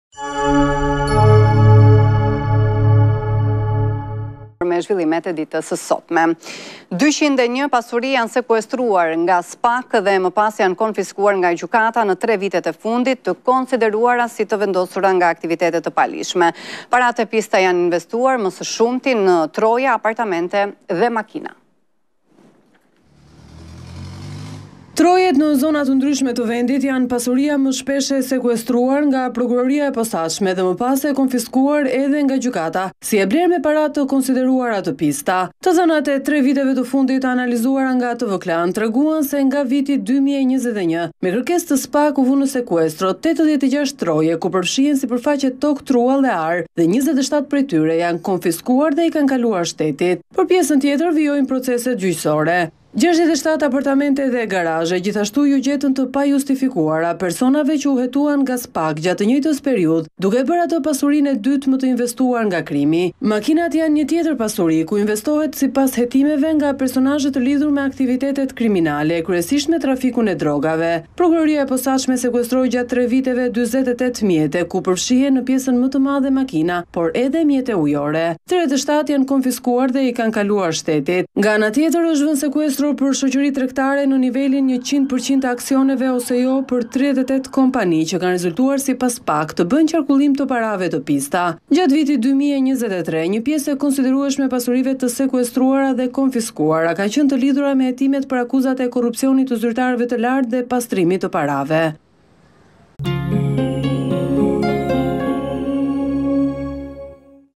Tv Klan - GJKKO bllokoi 201 pasuri të krimit në 3 vite
Trojet në zona të ndryshme të vendit janë pasuria më shpesh e sekuestruar nga Prokuroria e Posaçme dhe më pas e konfiskuar edhe nga Gjykata si e blerë me para të konsideruara të pista, thënë ndryshe të aktiviteteve kriminale.

Të dhënat e 3 viteve të fundit të analizuara nga Tv Klan treguan se nga viti 2021 me kërkesë të SPAK u vunë në sekuestro 86 troje, ku përfshihen sipërfaqe tokë truall e arë dhe 27 prej tyre janë konfiskuar dhe i kanë kaluar shtetit.

Për pjesën tjetër vijojnë proceset gjyqësore. 67 partamentet dhe garazhe gjithashtu iu gjetën të pajustifikuara personave që u hetuan nga SPAK gjatë të njëjtës periudhë, duke e bërë atë pasurinë e dytë më të investuar nga krimi.

Makinat janë një tjetër pasuri ku investohet sipas hetimeve nga personazhe të lidhur me aktivitete të kriminale, e kryesisht në trafikun e drogave. Prokuroria e Posaçme sekuestroi gjatë 3 viteve 48 mjete, ku përfshihen në pjesën më të madhe makina, por edhe mjete ujore. 37 janë konfiskuar dhe i kanë kaluar shtetit.

Nga ana tjetër është vënë sekuestro për shoqëri tregtare në nivelin 100% ose jo për 38 kompani që kanë rezultuar sipas SPAK të bëjnë qarkullim të parave të pista.

Gjatë vitit 2023, një pjesë e konsiderueshme e pasurive të sekuestruara dhe konfiskuara kanë qenë të lidhura me hetimet për akuzat e korrupsionit të zyrtarëve të lartë dhe pastrimit të parave.